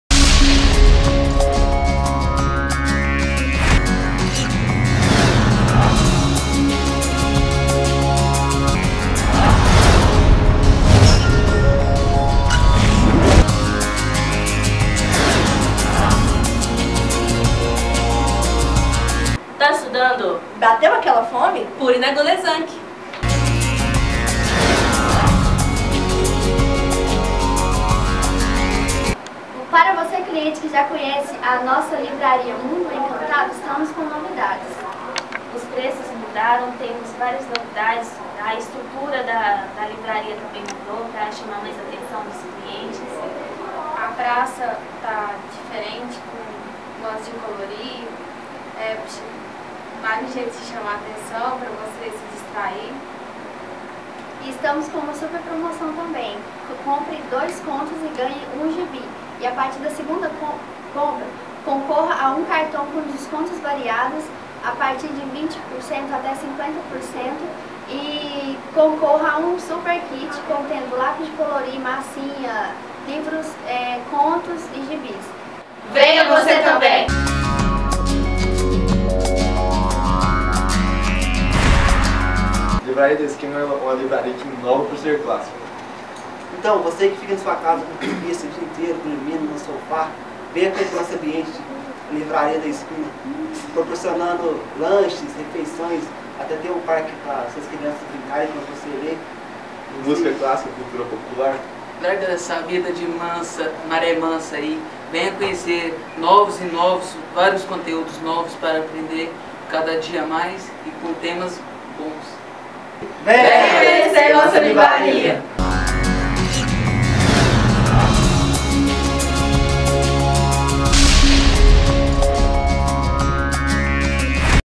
Faculdade Aberta ESAMC - E. E. João Rezende
Os alunos da E. E. João Rezende visitaram a ESAMC de Uberlândia, dia 14 de Abril de 2014, em uma manhã produtiva e divertida. Confira!